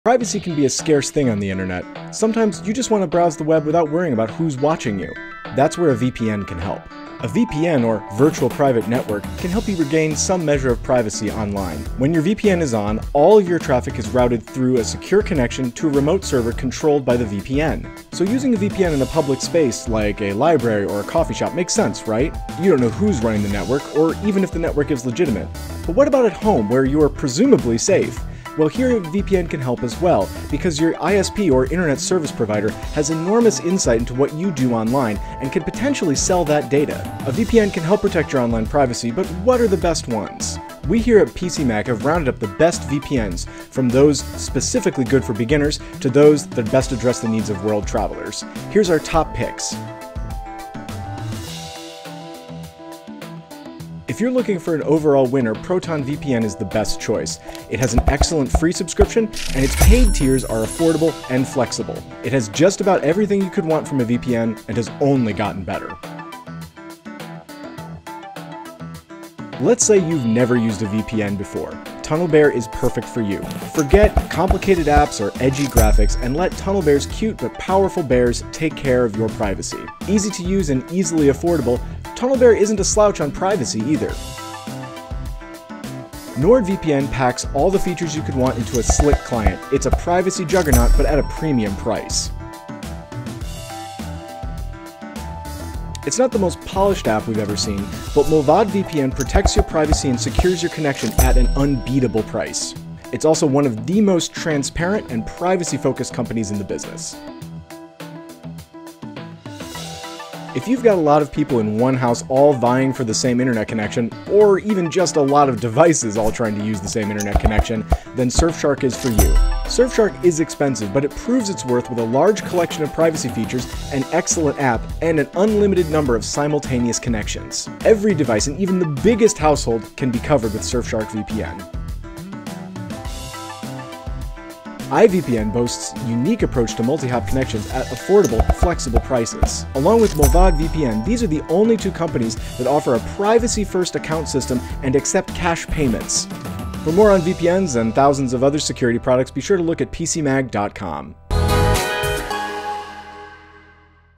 The Best VPN Services for 2022
A VPN, or virtual private network, is an easy way to improve your online privacy. We've reviewed scores of them, and these are the best VPN services we've tested.

00:00 Introduction
00:50 Best Overall VPN: ProtonVPN
01:07 Best VPN for First-Time Users: TunnelBear VPN
01:24 Best Premium Choice VPN: NordVPN
01:34 Best VPN for Bargain Hunters: Mullvad VPN
01:47 Best VPN for Large Households: Surfshark VPN
02:11 Best VPN for Privacy Hawks on a Budget: IVPN
02:26 Outro